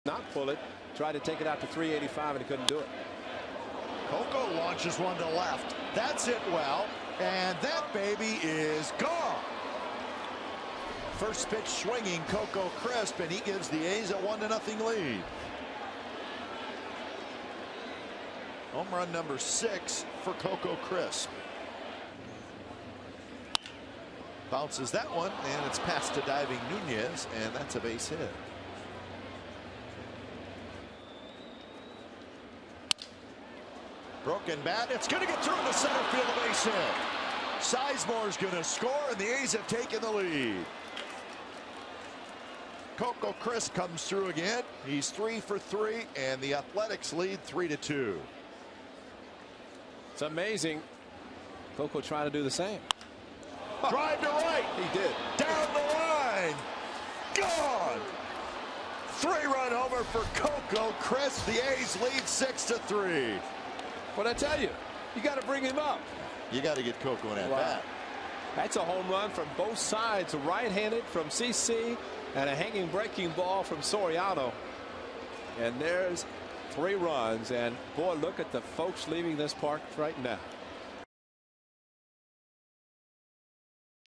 2011/08/24 Coco's big game
OAK@NYY: Coco collects four hits and five RBIs